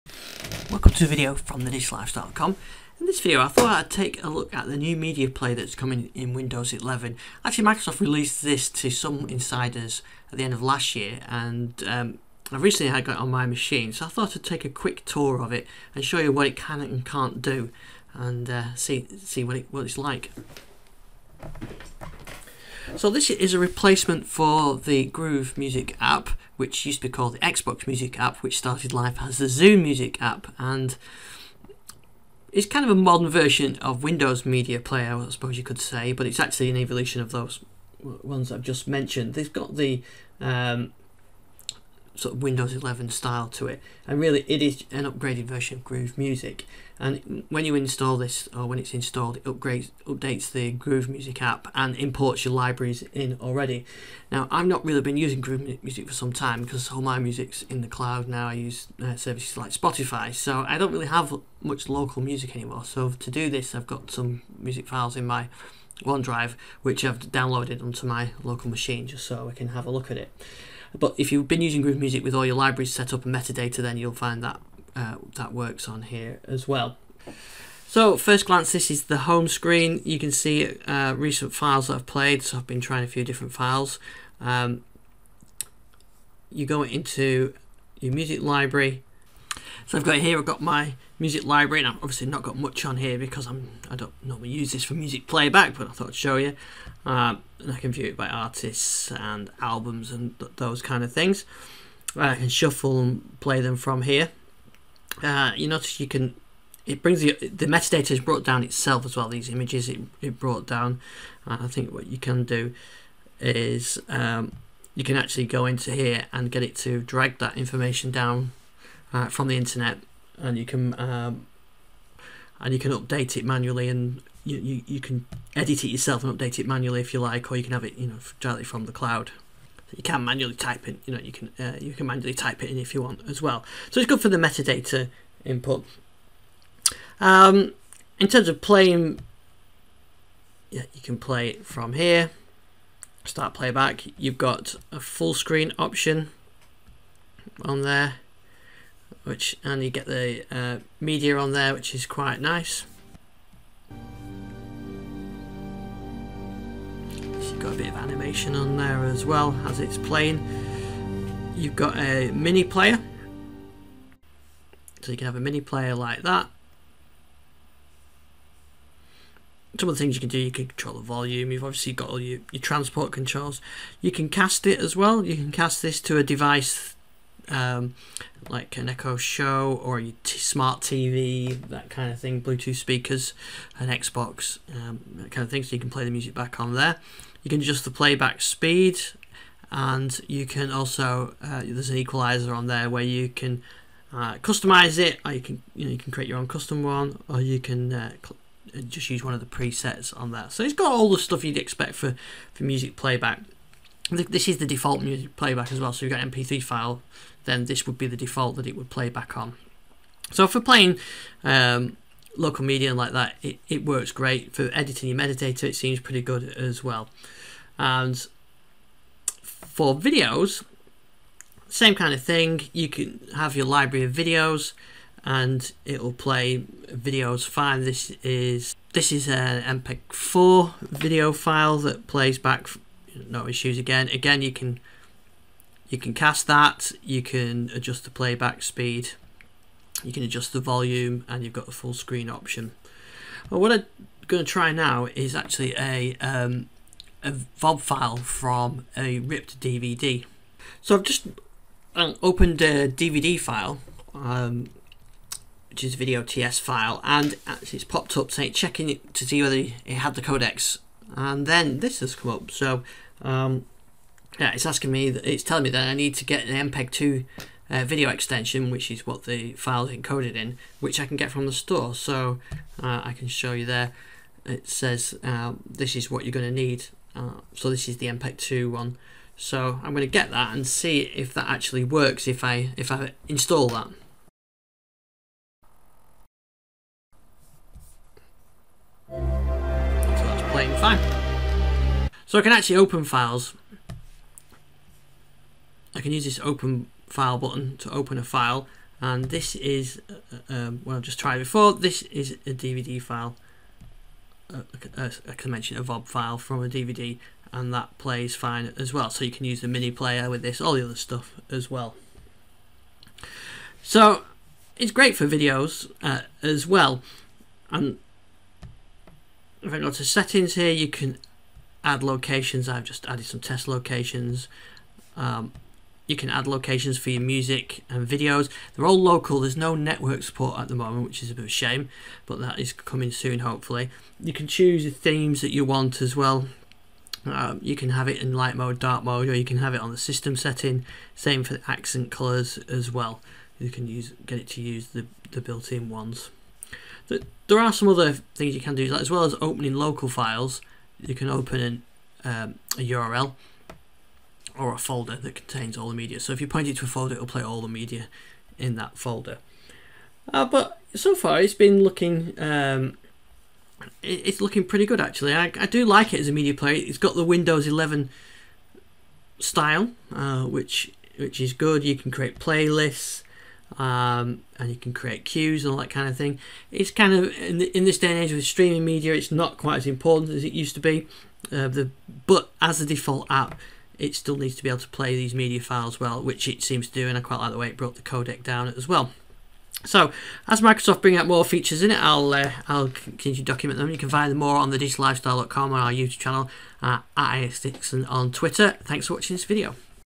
Hands on with the new Windows 11 Media Player app
At the end of last year Microsoft previewed its new Media Player app, replacing the old Groove Music app. The app that was once Zune Music, then Xbox Music became Groove Music and now is just Media Player.
The app is about to roll out to all Windows 11 users so here is my hands on look at the new media player application. In the video I look its music play back features, media management features and video playback. I also look at how it downloads codec from Microsoft Store on demand.